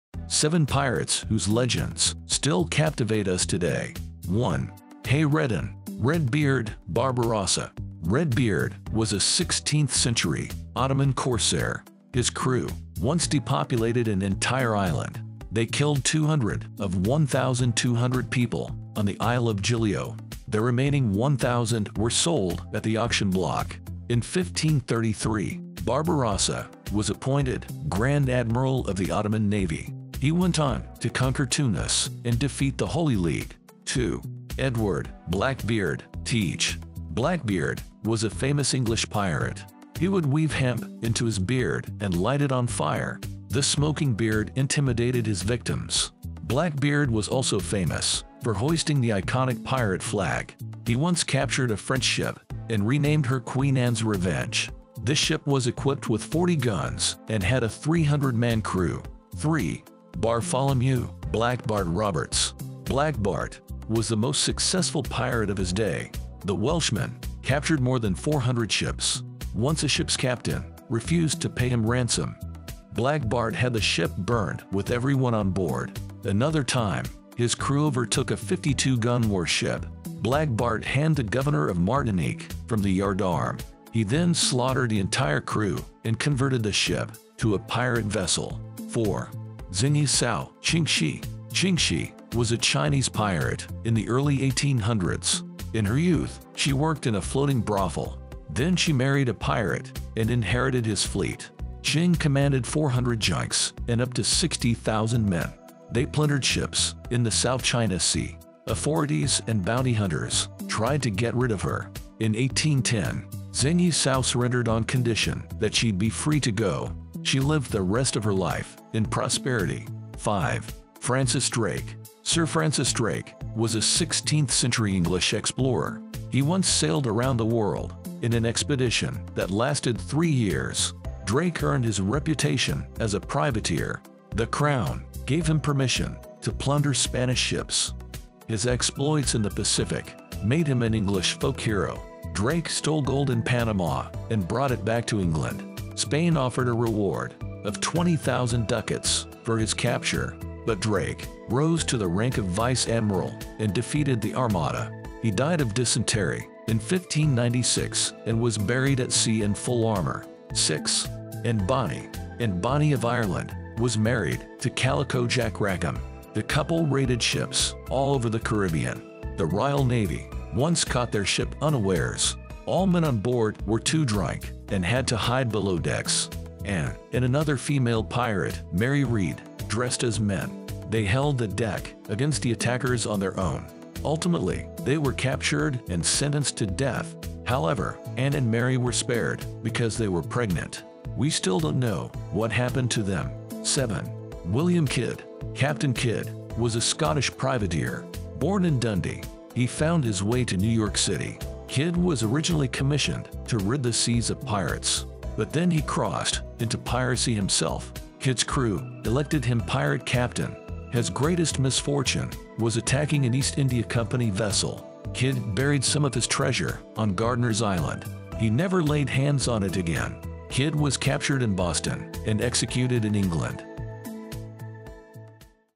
7 Pirates Whose Legends Still Captivate Us Today
If you’ve seen any of the Pirates of the Caribbean movies, you will recognize some of these real-life characters...